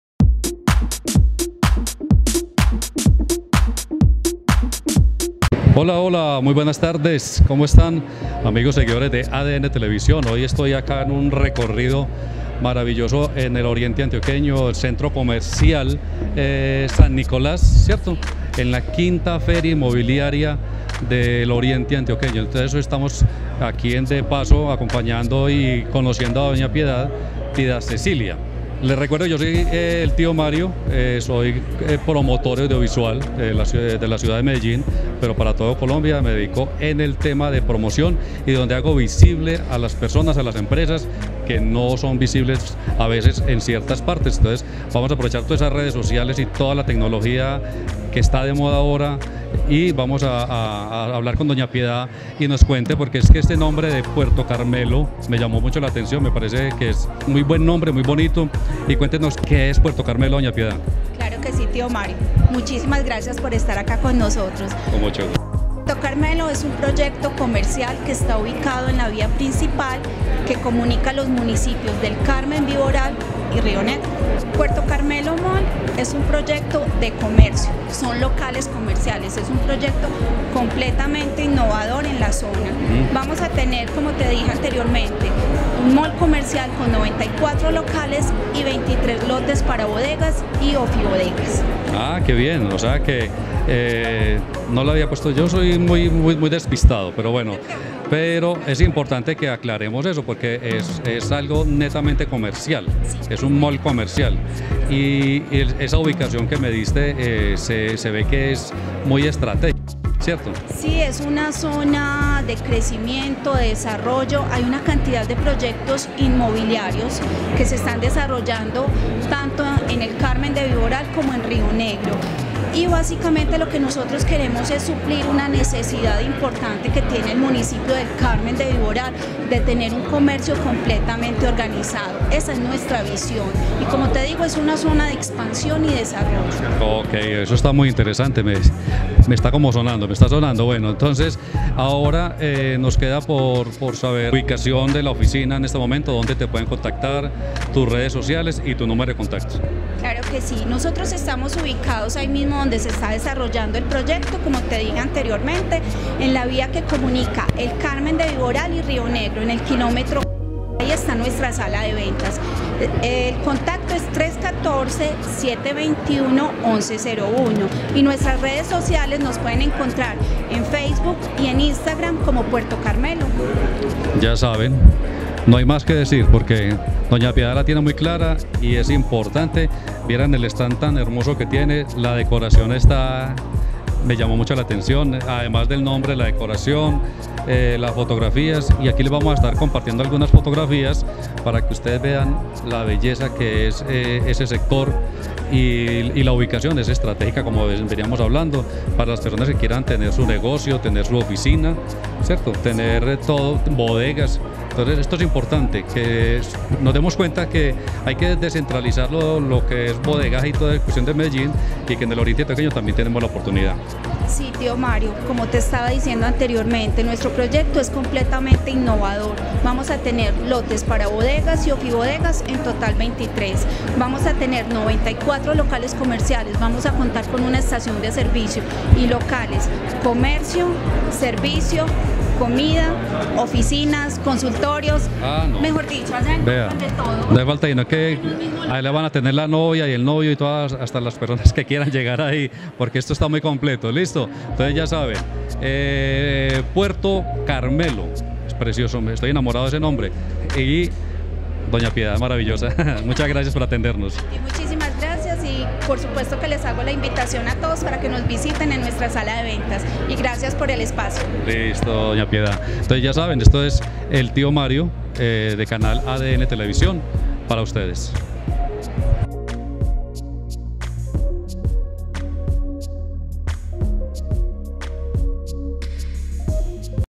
EL TIO MARIO EN ENTREVISTA - PUERTO CARMELO
ALIANZA DE MEDIOS Y EL TIO MARIO, EN ÉSTE MAGNÍFICO RECORRIDO EN EL CCIAL SAN NICOLAS en el municipio de Rionegro Antioquia. Entrevista a la señora PIEDAD CECILIA Directora de ventas de esta importante empresa y proyecto